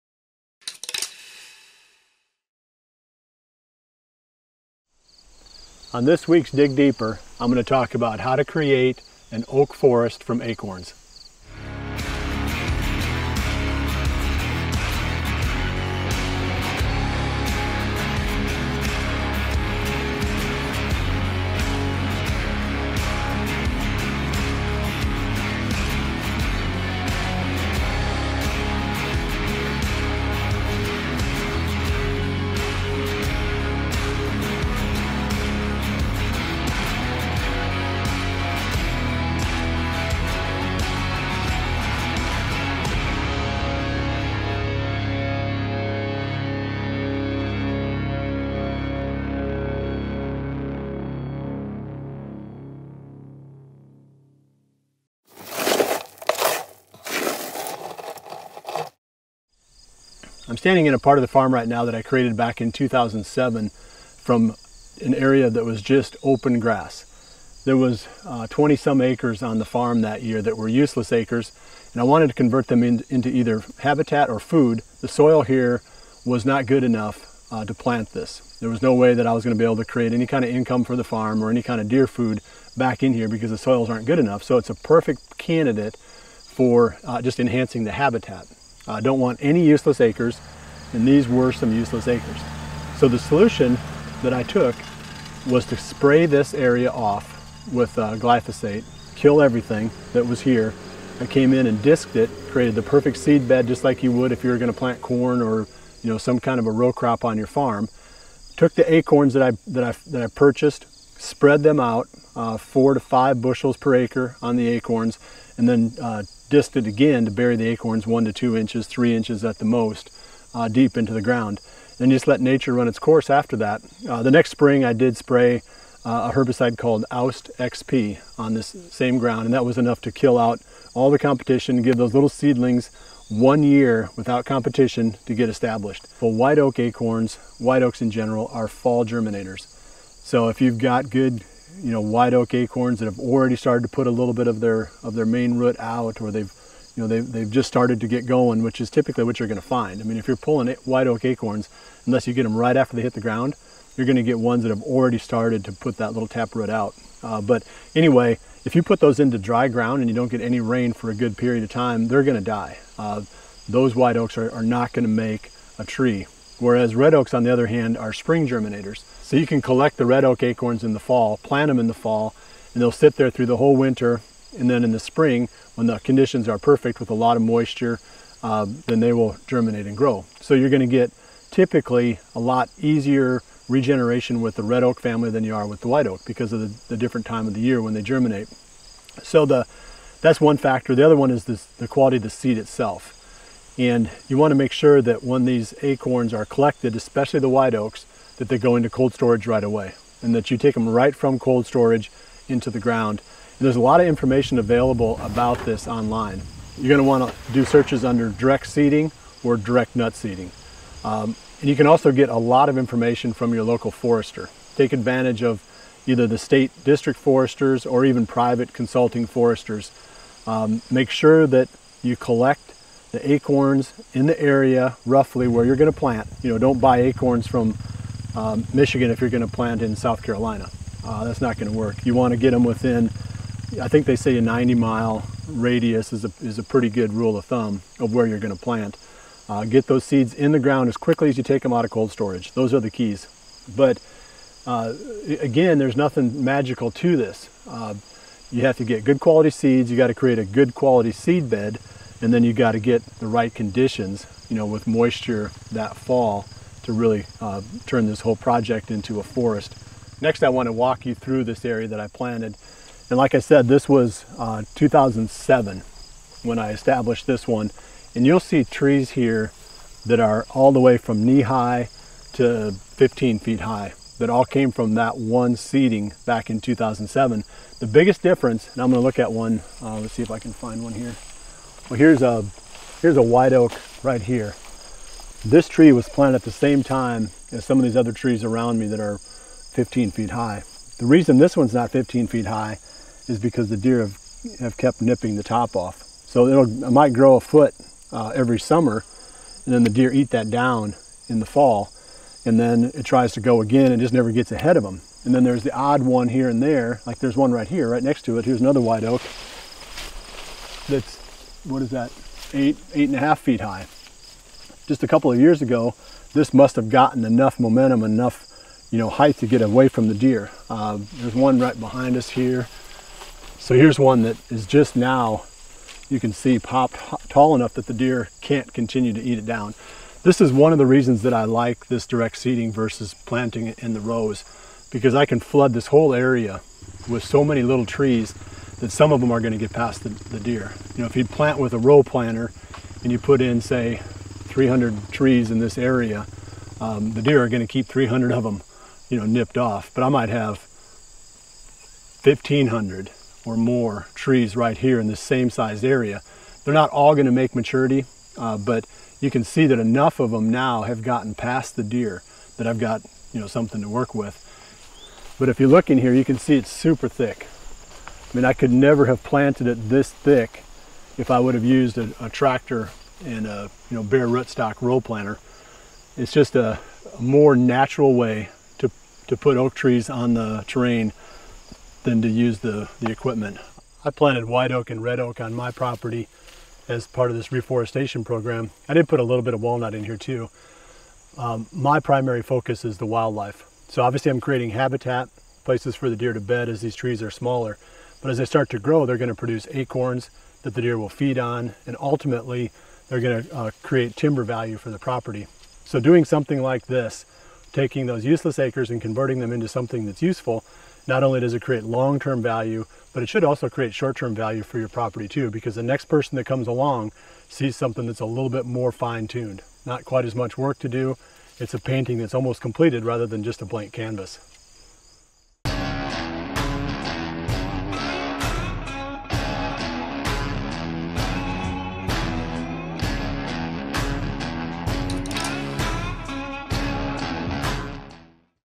How to Grow Oak Trees from Acorns
Bill Winke takes a look back at a 2007 project, and explains how to establish new oak seedlings from acorns